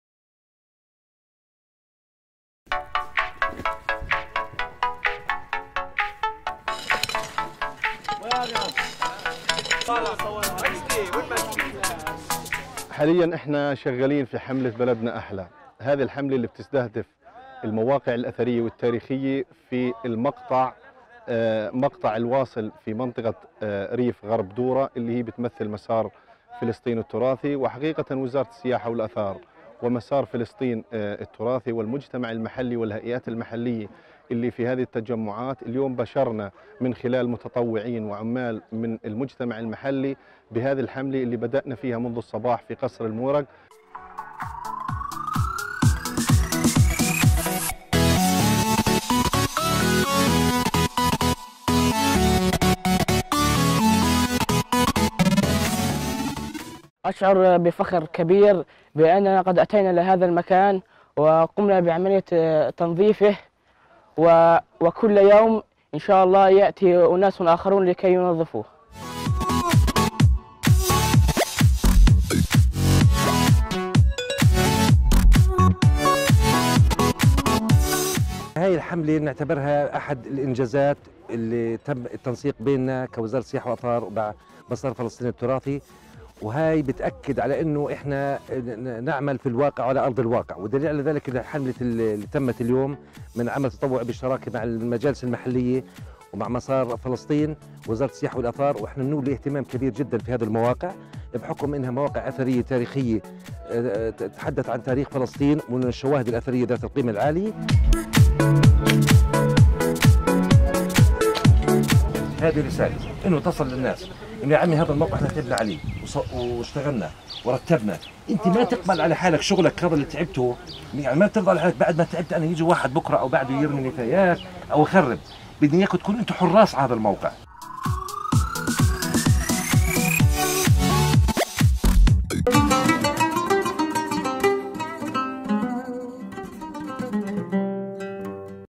Clean-Up Activity At Al-Muraq Palace
فيديو قصير  يُظهر نشاط تنظيف قصر المورق في قرية الكوم ضمن حملة "بلدنا أحلى" التي ينفذها اتحاد مسار فلسطين التراثي ببالتعاون مع وزارة السياحة والآثار الفلسطينية ,كجزء من مشروع CROSSDEV - المسارات الثقافية للتنمية الاجتماعية والاقتصادية المستدامة في البحر الأبيض المتوسط، بتمويل من الاتحاد الأوروبي من خلال برنامج ENI CBC MED.
A short video showing the clean-up activity at Al-Muraq Palace in the village of Al-Kum within “Baladna Ahla" cleaning campaign, the Palestine Heritage Trail Union implemented in cooperation with the Palestinian Ministry of Tourism and Antiquities, as part of CROSSDEV - Cultural Routs for Sustainable Social and Economic Development project in the Mediterranean, funded by the European Union through the ENI CBC MED programme.